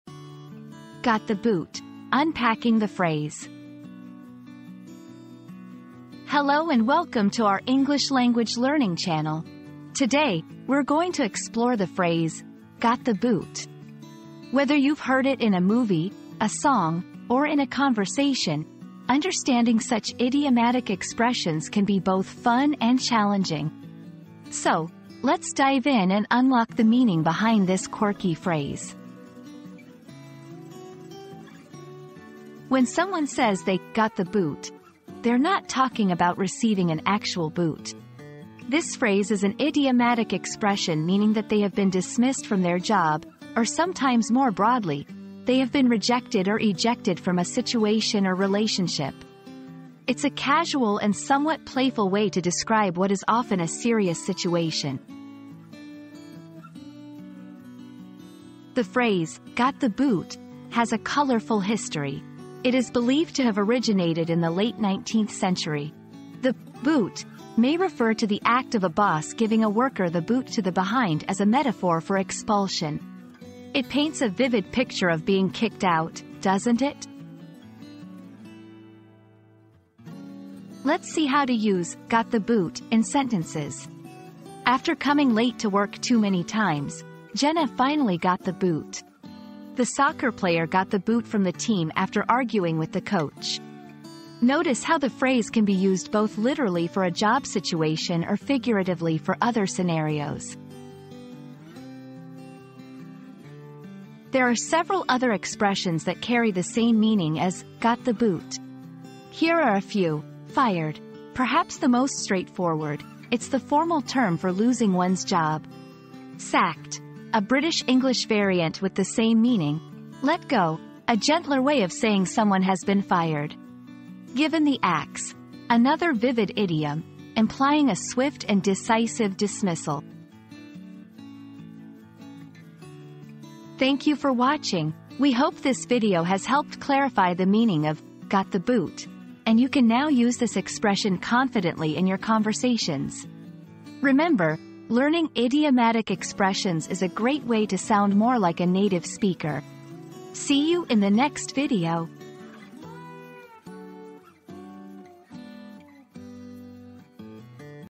Got The Boot: Unpacking the Phrase
Got The Boot: Unpacking the Phrase • Discover the fascinating origins and meanings behind the commonly used phrase 'Got The Boot' in this intriguing video. From its historical roots to its modern-day usage, delve into the intriguing world of idiomatic expressions and learn something new today!

00:00 • Introduction - Got The Boot: Unpacking the Phrase
00:32 • What Does "Got the Boot" Mean?
00:58 • Origin of the Phrase
01:20 • Using "Got the Boot" in Sentences
01:43 • Similar Expressions